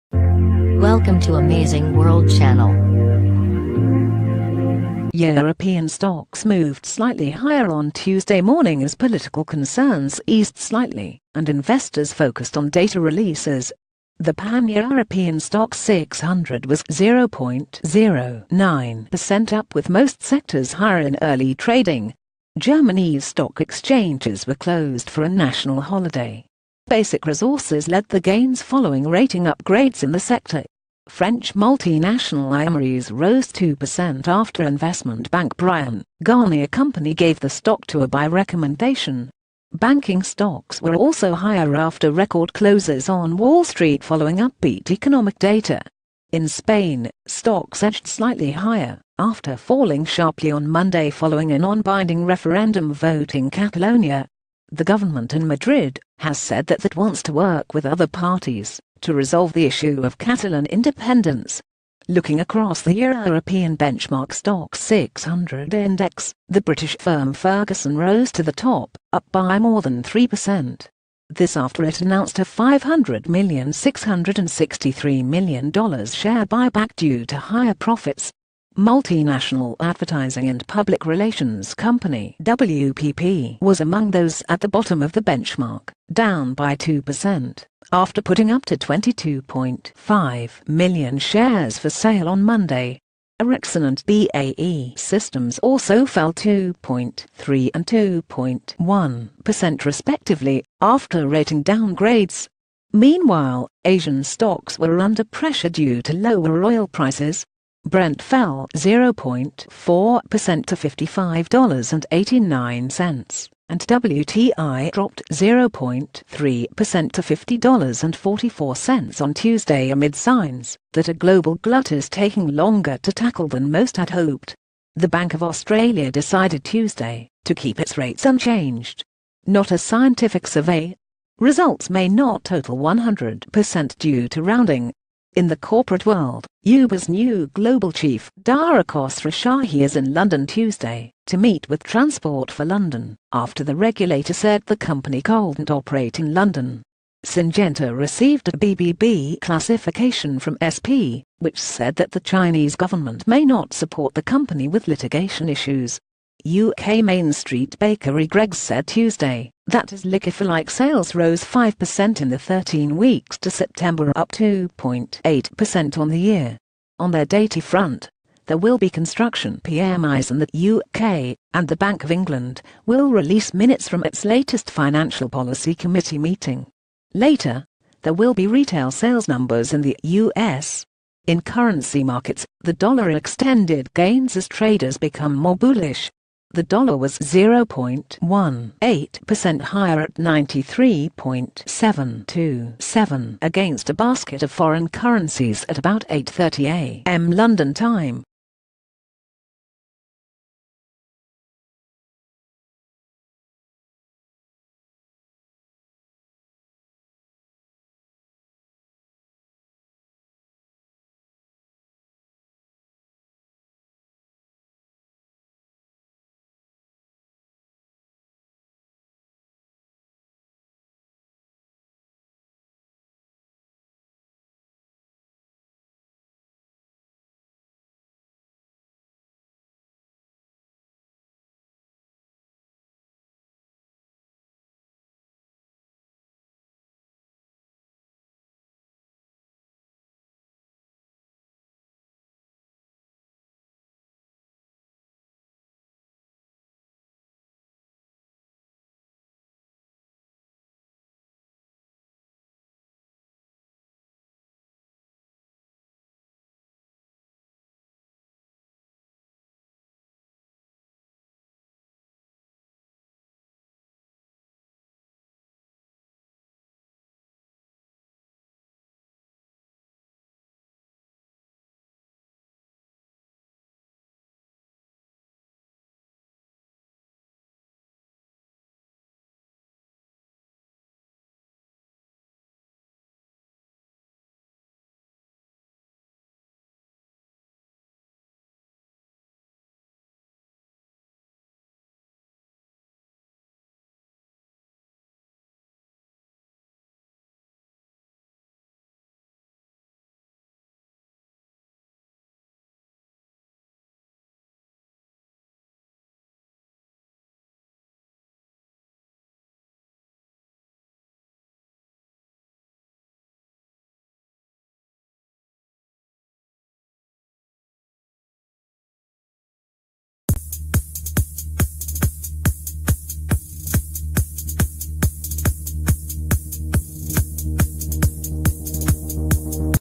European markets higher as spanish stocks rebound after catalonia concerns News Today
European markets higher as spanish stocks rebound after catalonia concerns
European stocks moved slightly higher on Tuesday morning as political concerns eased slightly and investors focused on data releases. The panEuropean Stoxx 600 was 0.09 percent up with most sectors higher in early trading. Germanys stock exchanges were closed for a national holiday. Basic resources led the gains following rating upgrades in the sector. French multinational Imerys rose 2 percent af...